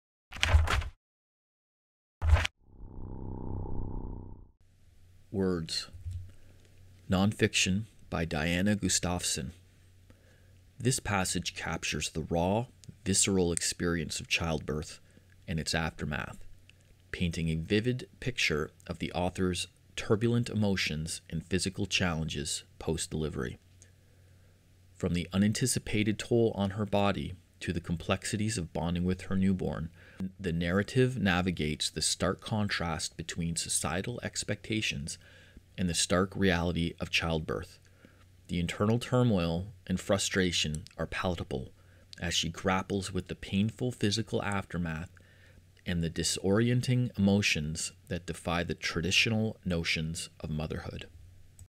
"Words" by Diana Gustafson • Short Story Synopsis - Dreamers Issue 16
“Words were minced into morsels that she could spit across the room like outrageous profanities."

More of our author post series Featured in issue 16 of Dreamers Magazine, with Diana Gustafson's story "Words" available

Featured in issue 16 of Dreamers Magazine, this story and many others can be purchased

Are you an author?